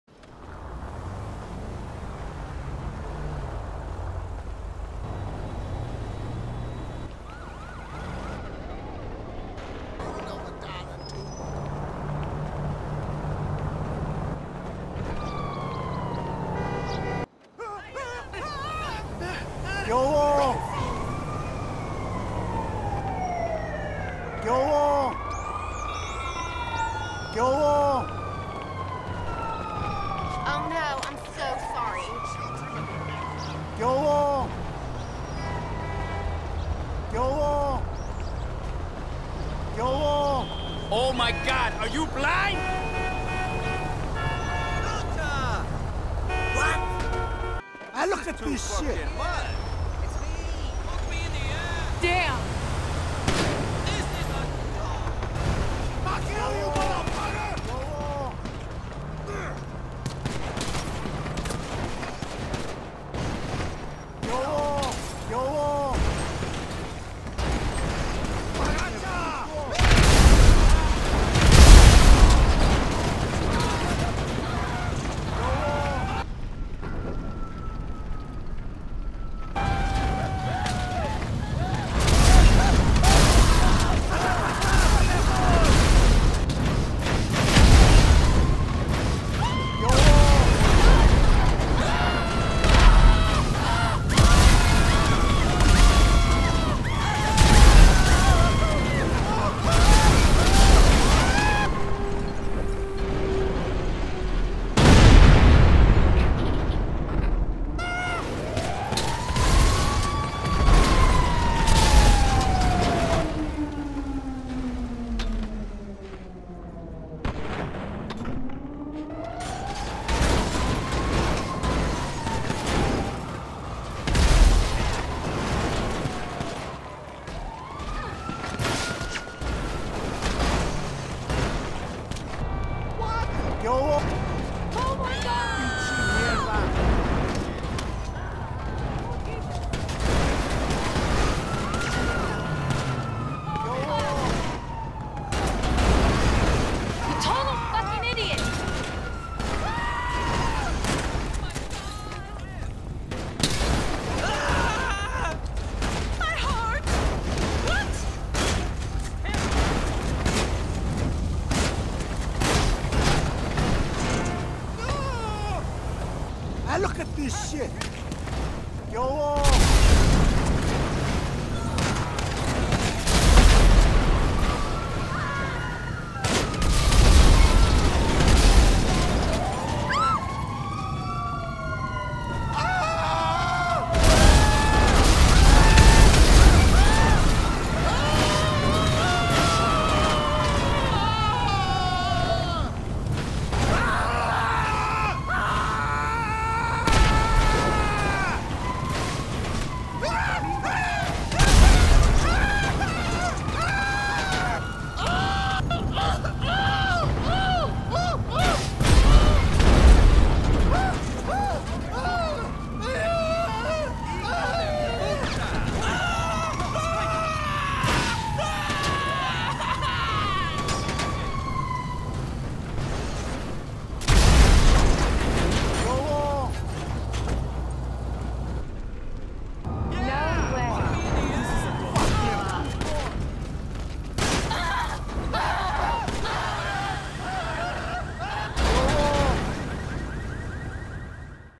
Demented Crime Boss*
All this Crime Lord knows how to do is say "YO LONG" and blow up endless cars.